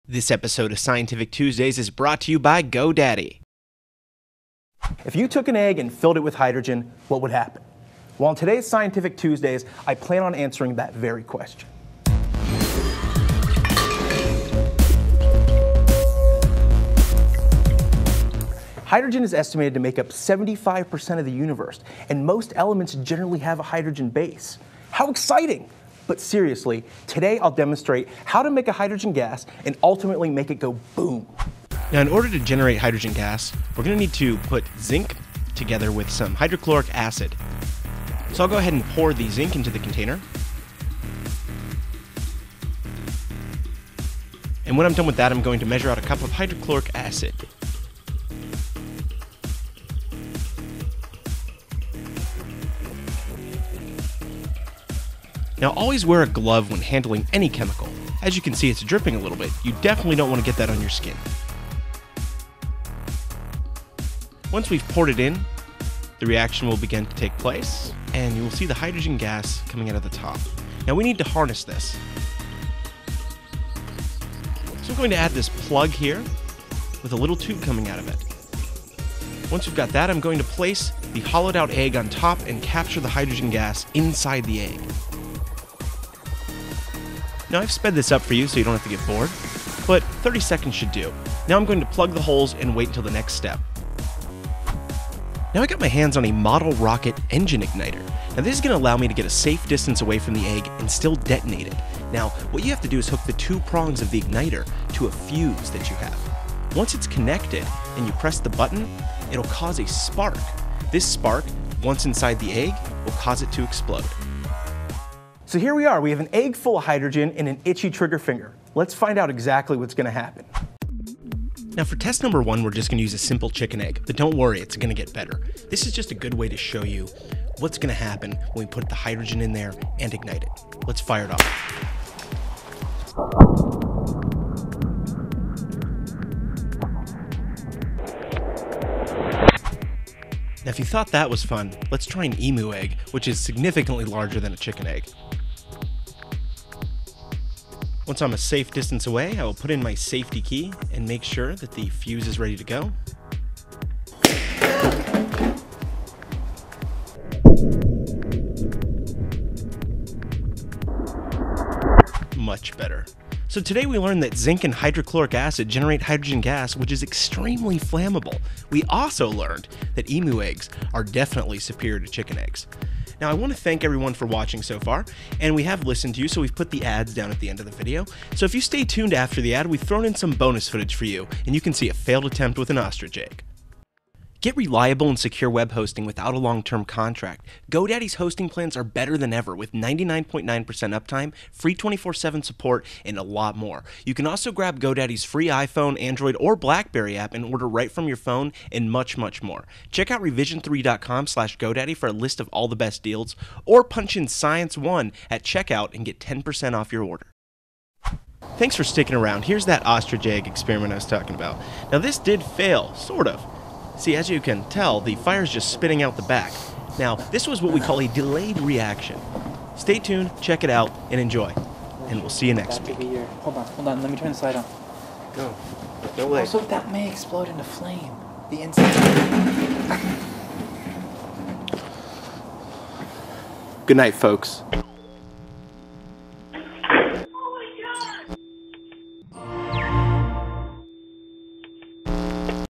Scientific Tuesdays - Eggsplosion!
On this week's episode of Scientific Tuesdays, we take some hydrogen gas, an egg, and a little fire and find out what happens when you combine the three.

Note: In the spirit of household items, we created the bottle plug by wrapping duct tape around a wad of rolled-up paper towels. We simply placed a tube from a pen in the center and wrapped about 12 times, this works great.

Remember, these demonstrations are attempted by a professional and should not be attempted at home. But if you don't want to listen to us and throw caution to the wind, always wear proper safety gear such as gloves, masks, and protective clothing.

for a list of all of the
amazing GoDaddy deals from Revision3. Make sure to scroll down to Scientific Tuesdays so they know we sent you! .